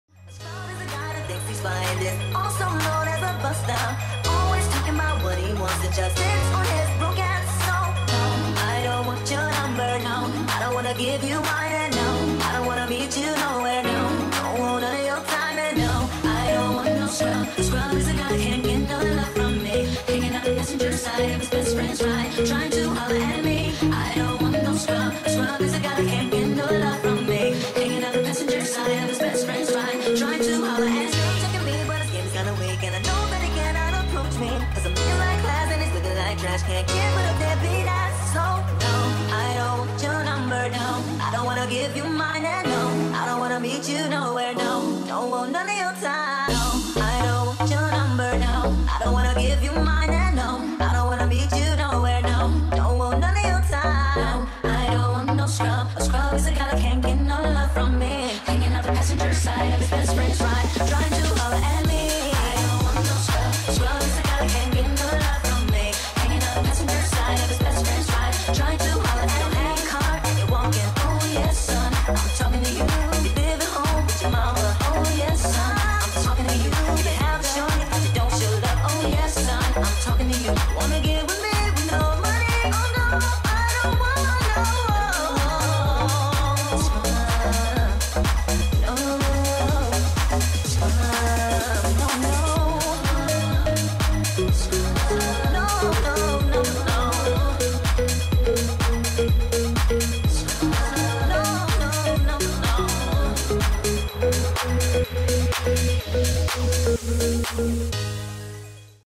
A Tamera Mowry Inspired Hairstyle with Curl Keeper Products
Hey Curlies! Noelia (@curleila) used Curl Keeper Leave in Conditioner, Curl Keeper Styling Cream, Curl Keeper Gel, and the Flexy Brush to recreate  @tameramowrytwo inspired hairstyle on the @therealdaytime Premier show!